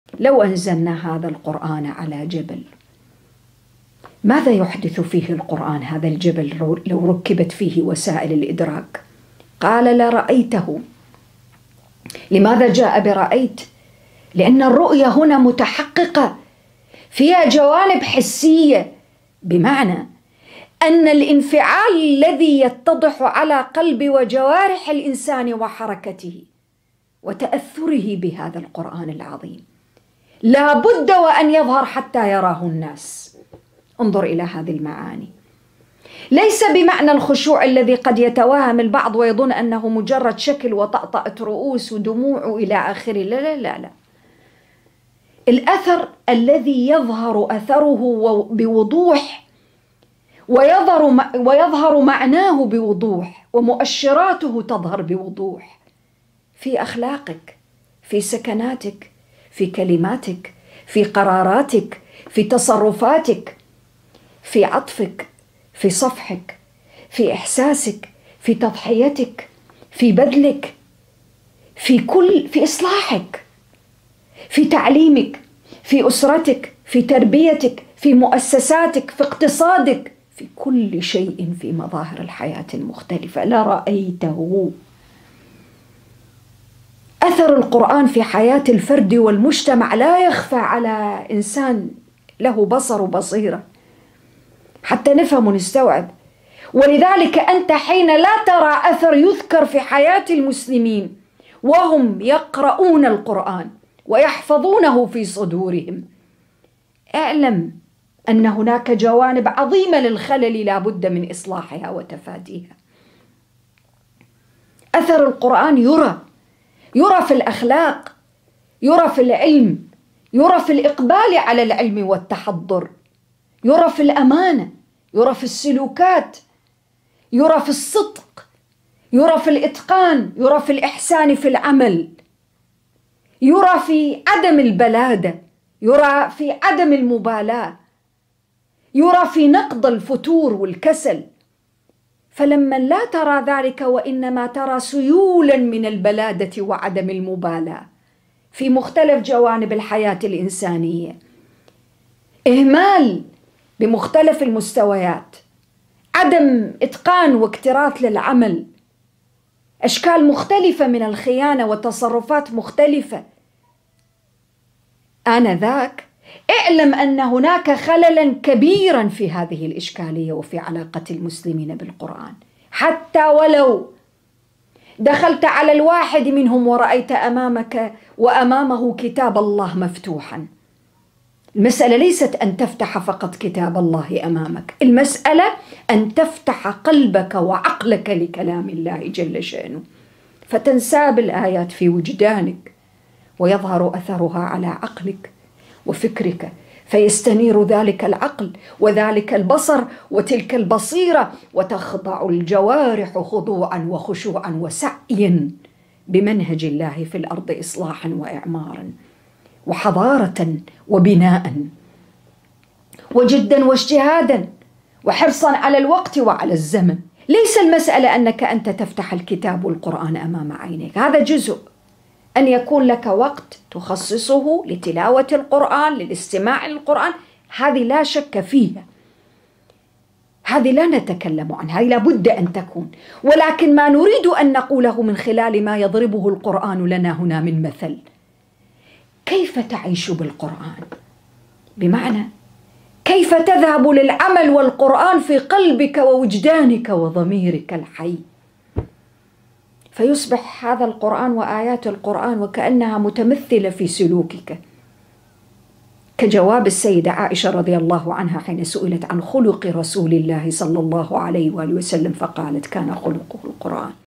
لو أنزلنا هذا القرآن على جبل: مقطع قصير من تدبر سورة الحشر 14
الحسابات الرسمية لـ أ.د. رقية طه العلواني